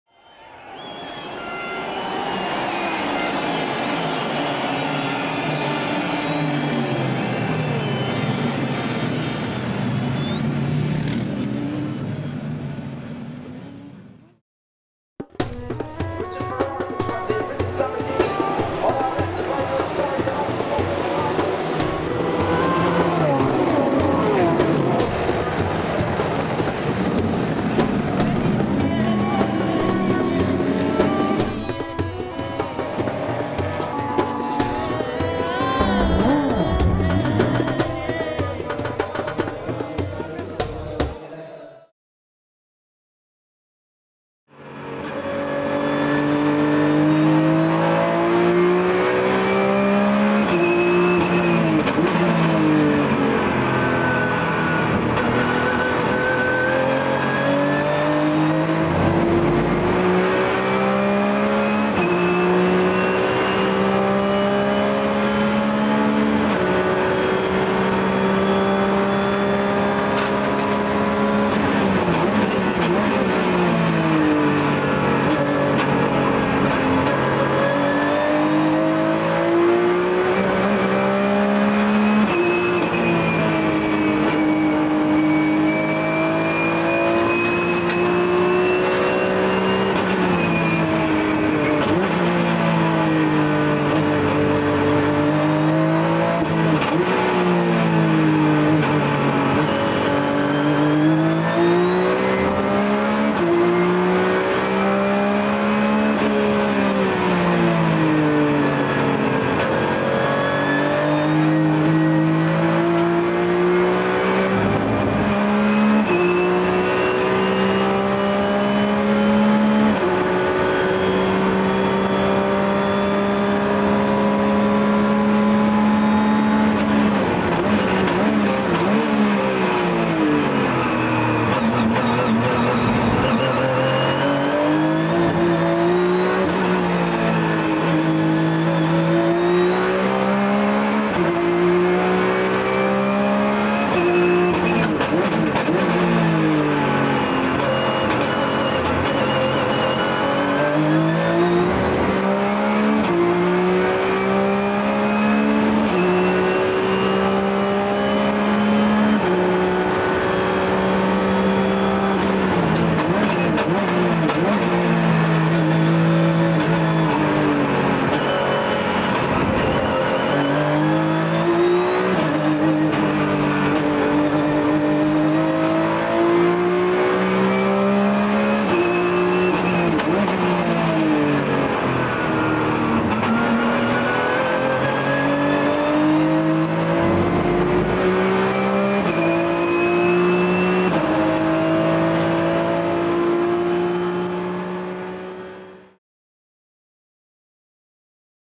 24H Dubai 2012 Onboard race&event Car Collection 1 Porsche 997 GT3 Cup-S Qualification
24H Dubai 2012 Onboard race&event Car Collection 1 Porsche 997 GT3 Cup-S / Qualification - Driver: Elmar Grimm (c)race-media.tv